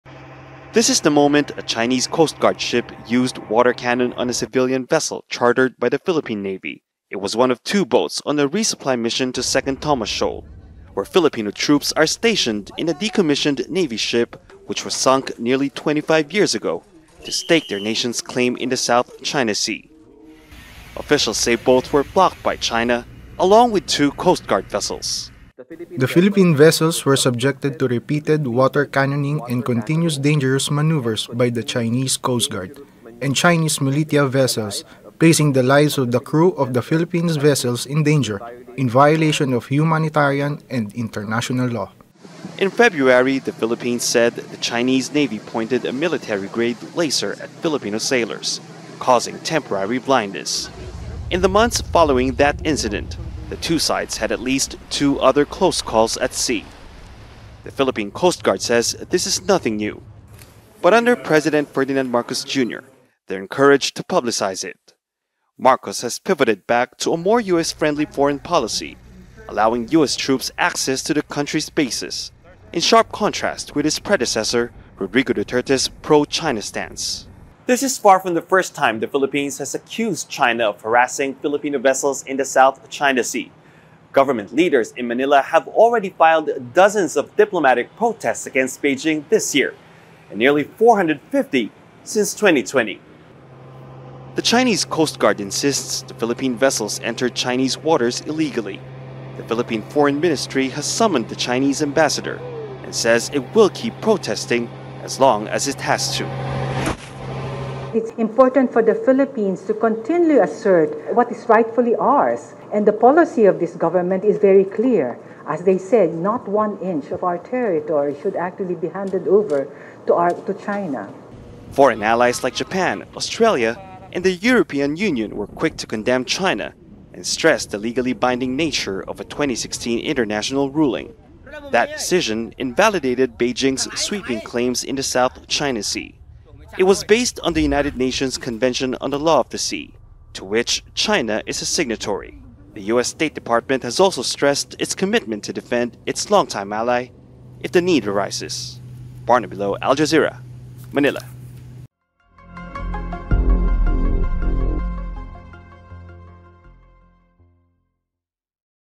Philippines summons China envoy after water cannon incident in disputed sea
Manila has summoned Beijing’s ambassador a day after Chinese coastguards blocked and used water cannon on Philippine vessels in the disputed South China Sea, according to President Ferdinand Marcos Jr.
“Our secretary of foreign affairs summoned Ambassador Huang today and gave him a note verbale, including pictures, video about what happened and we are awaiting their reply,” Marcos told reporters on Monday.
The Philippines on Saturday accused China’s coastguard of using water cannon to block its vessels carrying food, water, fuel and other supplies for its military personnel stationed at Second Thomas Shoal in the Spratly Islands.

Al Jazeera’s Barnaby Lo reports from Manila.